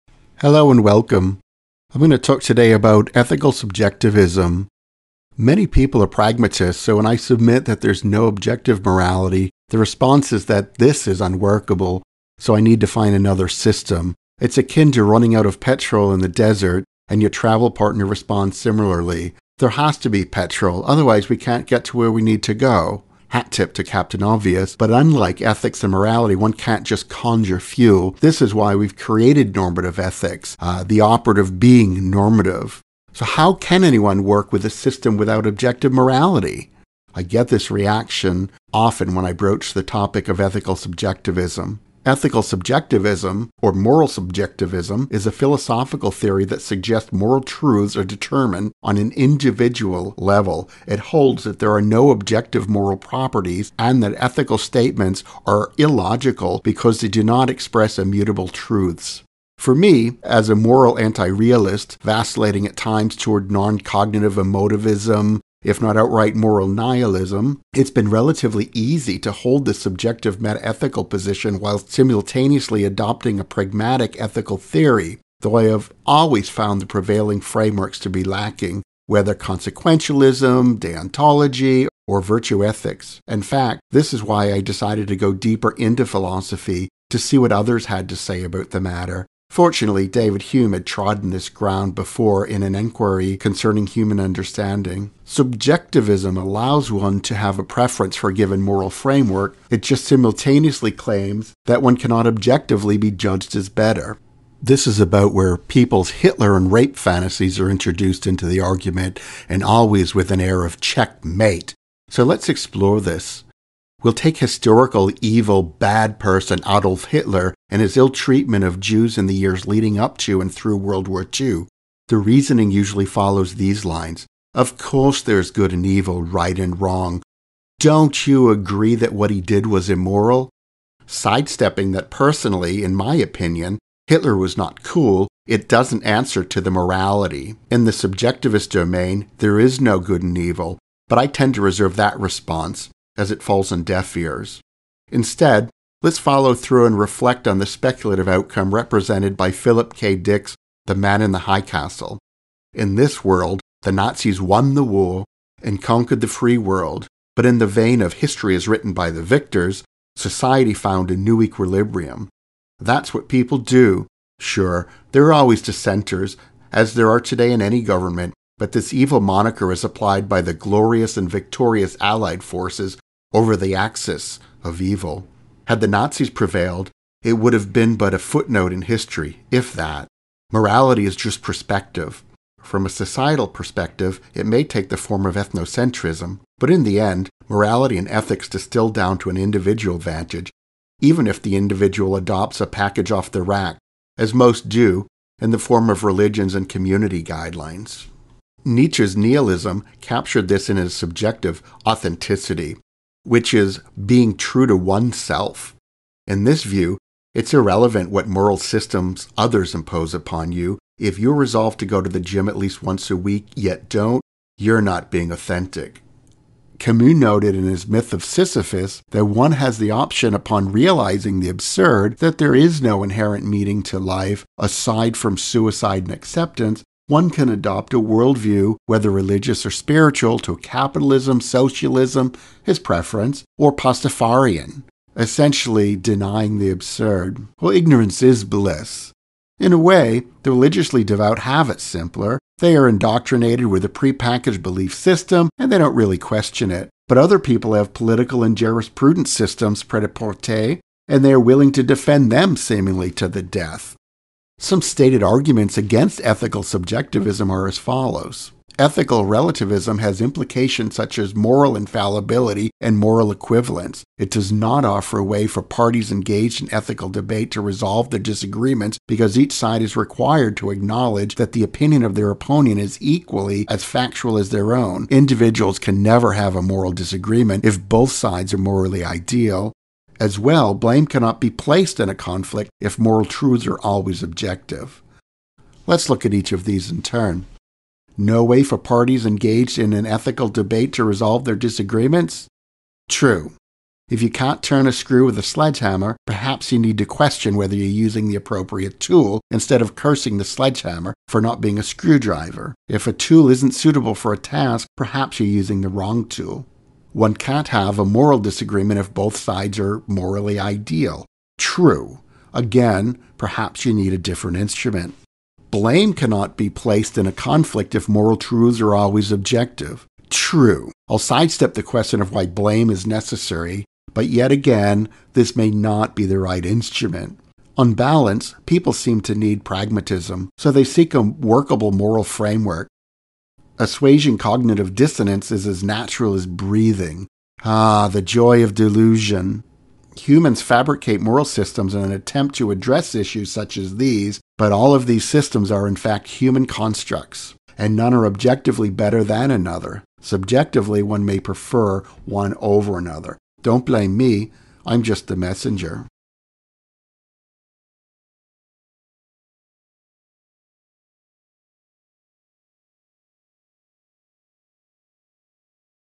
Ethical Subjectivism
A short-ish perspective on reactions to ethical subjectivism.

This YouTube channel is an extension of the Philosophics blog where you can find related content and a transcript for this video.